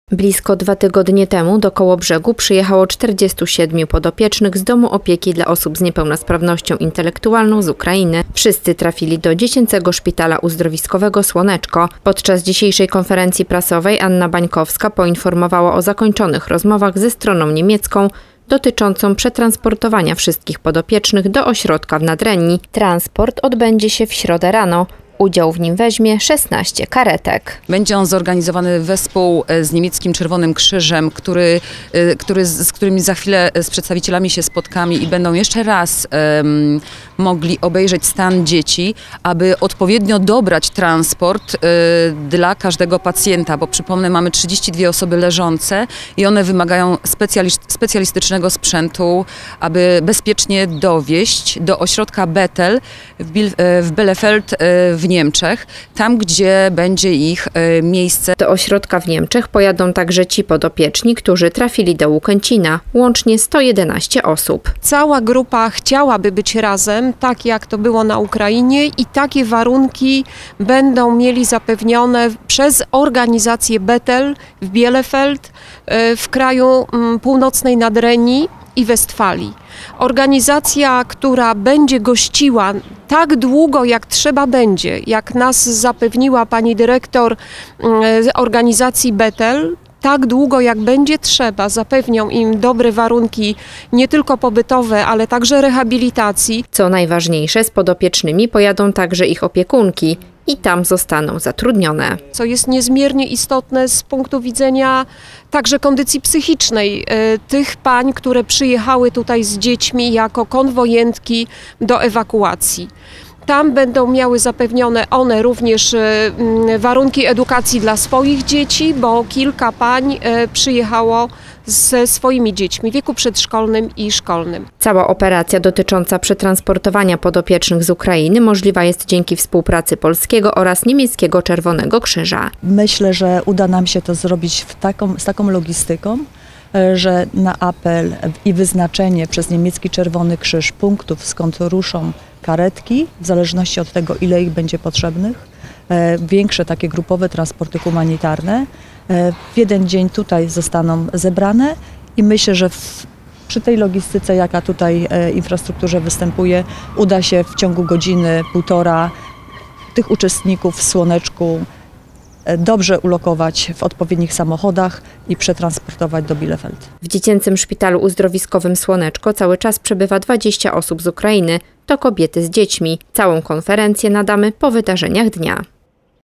NIEPEŁNOSPRAWNE DZIECI ZE SŁONECZKA POJADĄ DO NIEMIEC
Niepełnosprawni podopieczni, którzy dwa tygodnie temu przyjechali z Ukrainy do Dziecięcego Szpitala Uzdrowiskowego „Słoneczko” wkrótce zostaną przetransportowani do Niemiec. Podczas dzisiejszej konferencji prasowej Anna Bańkowska poinformowała o szczegółach ich wyjazdu.